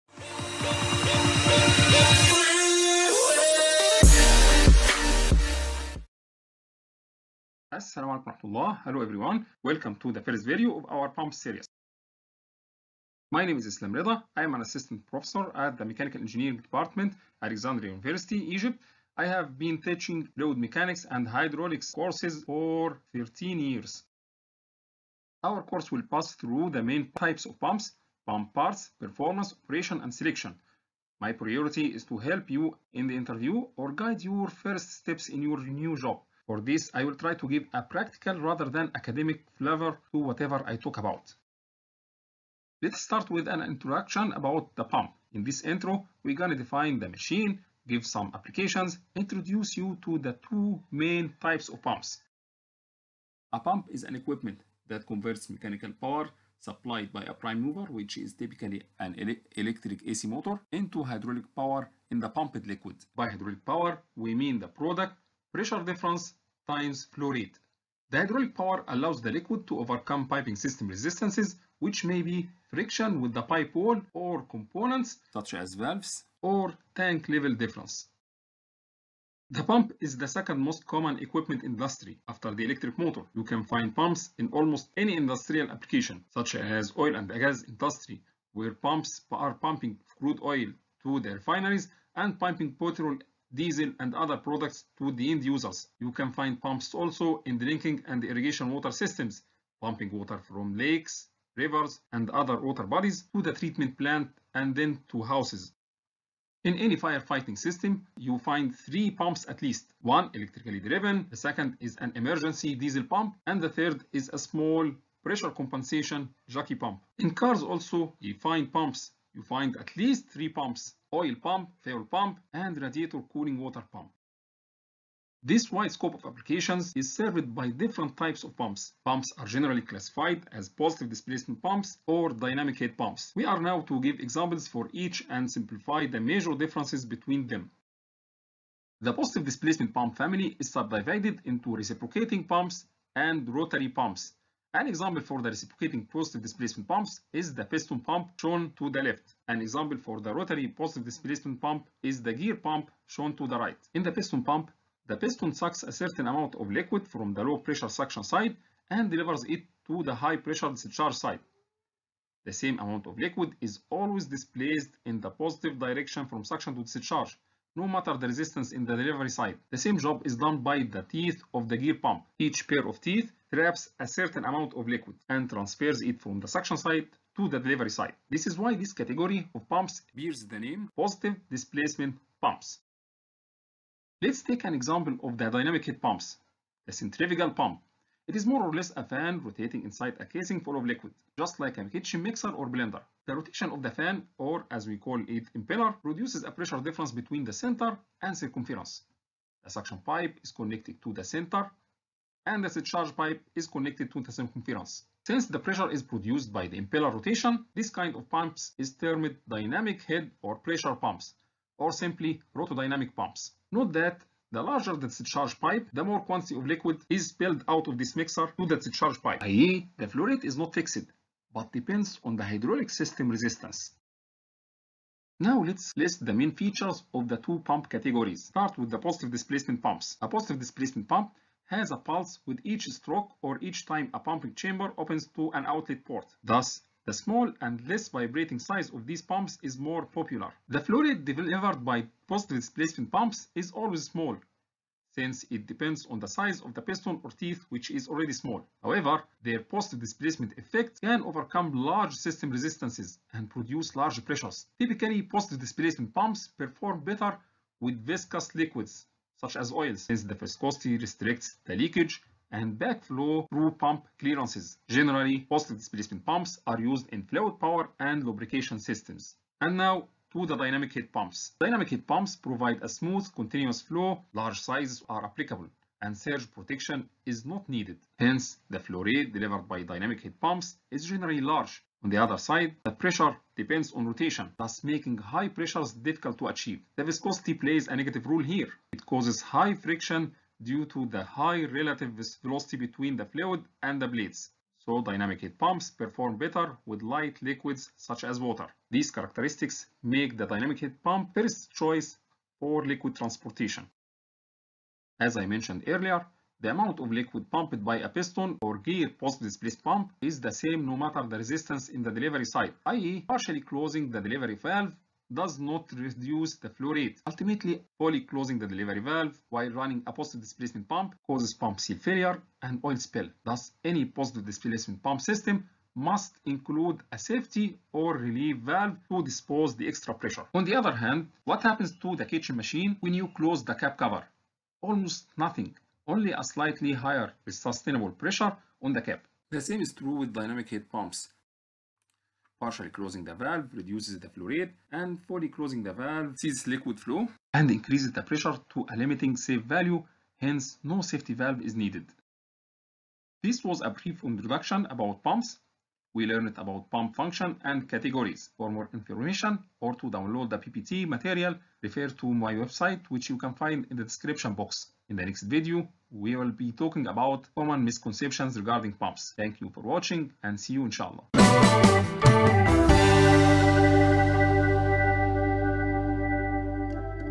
1 Introduction to pumps [En]
Introduction to pumps
What is a pump
Where to find pumps

Types of pumps
Positive displacement pumps vs dynamics head pumps